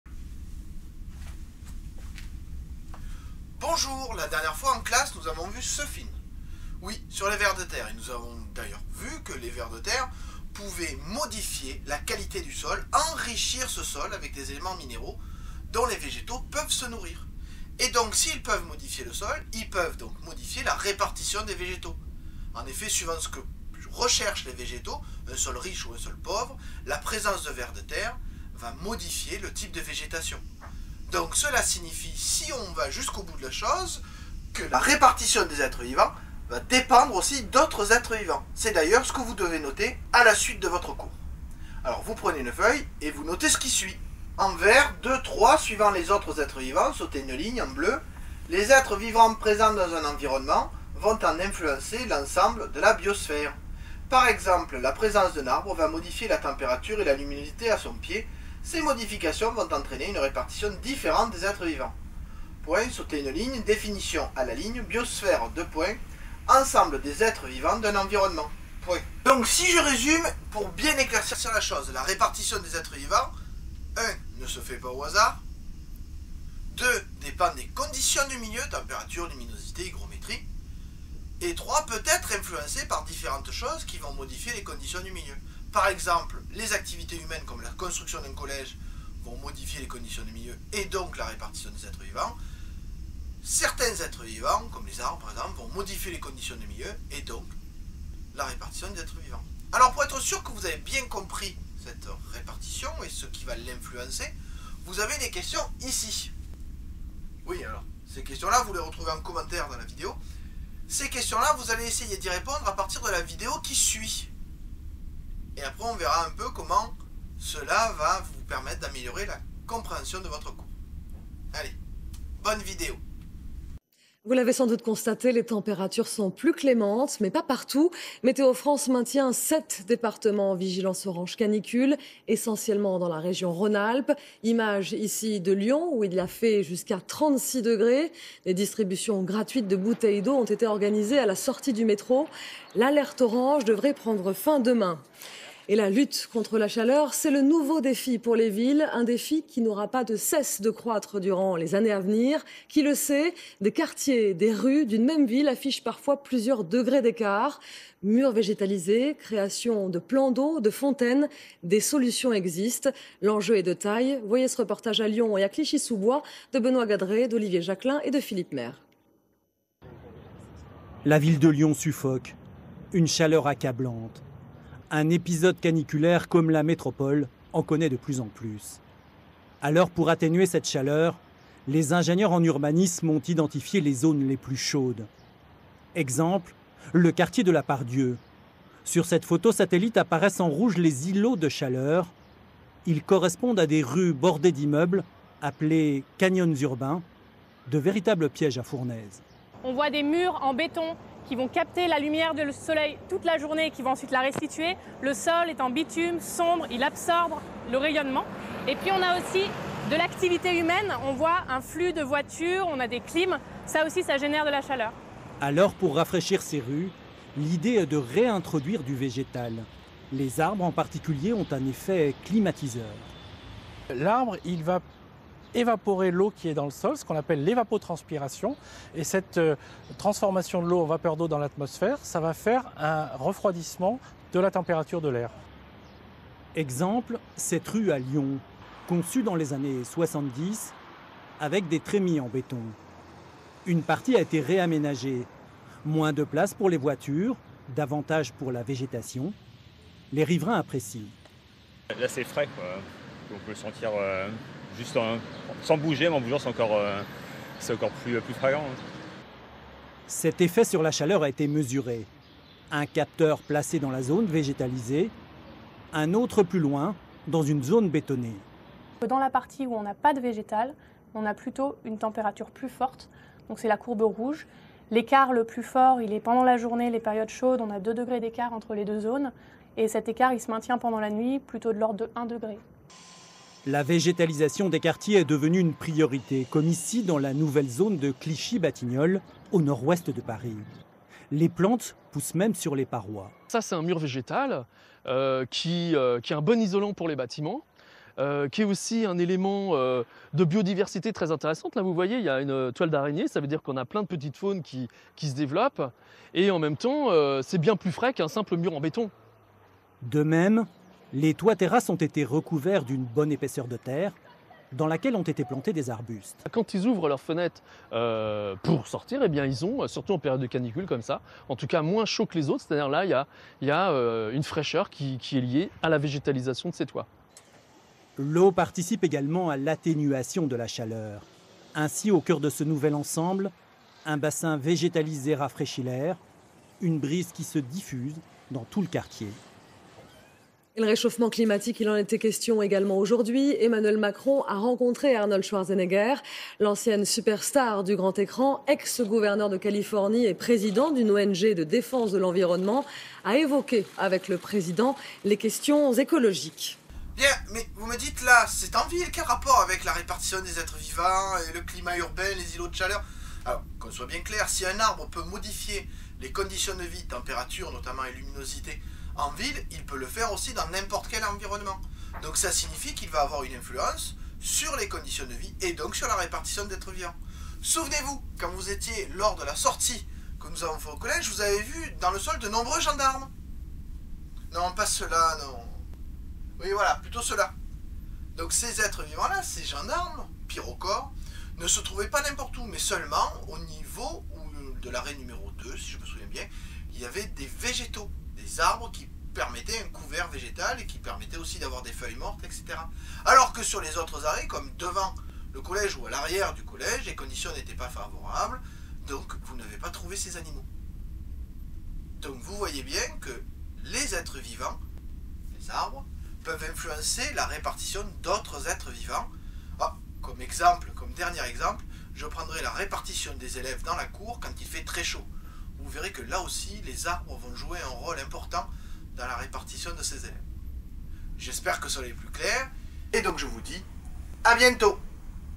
6ème Chapitre 01 Leçon 01 cours 04 : influence des êtres vivants sur les conditions de milieu.
Voilà une première vidéo pour mes 6èmes.
Les questions à faire (si vous le voulez, pour vous entrainer) sont :
Q1 : de quelle ville s’agit il ?
Q2 : qu’est ce qu’un canyon urbain ?
Q3 : pourquoi un canyon urbain est il chaud ?
Q4 : Quel est l’effet des arbres sur la température d’une ville ?
Q5 : Comment les arbres agissent ils sur la température d’une ville ?

Bon travail ;-)